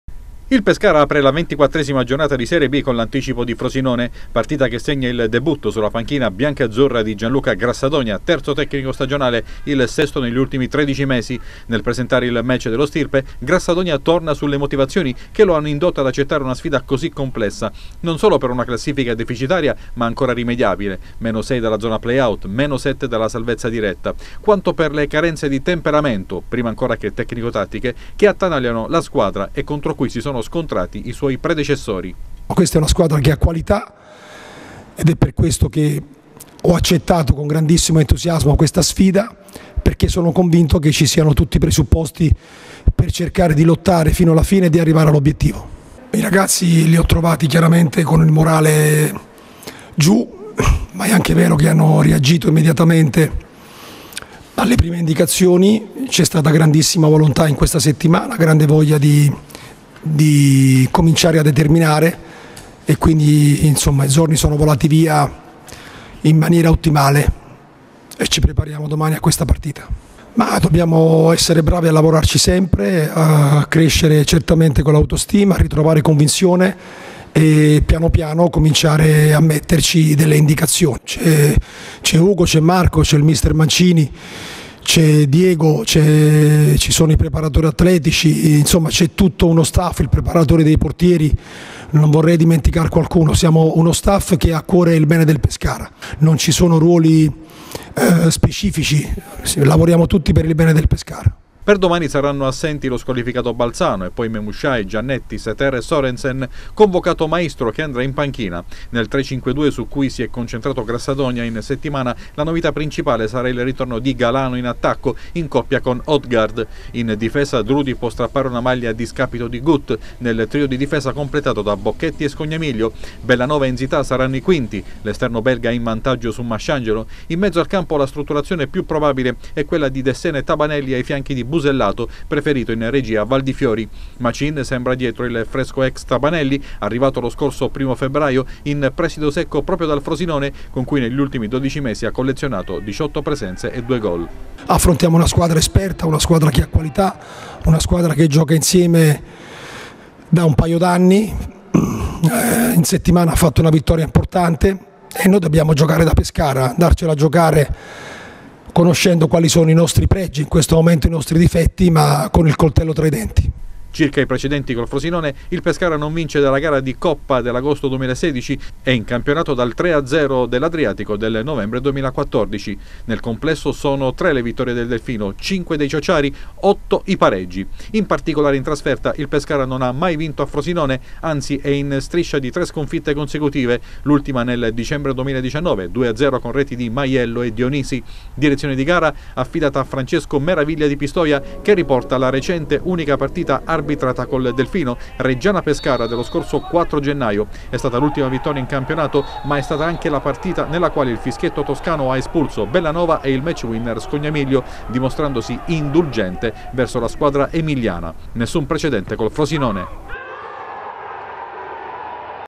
Debutta Grassadonia: “Pescara col coltello tra i denti”
Domani sera alle 21:00 il Pescara giocherà in trasferta a Frosinone. Non c’è tempo da perdere per il nuovo allenatore Gianluca Grassadonia. C’è la necessità di fare punti per dare inizio alla difficilissima impresa salvezza. Direzione di gara affidata a Meraviglia di Pistoia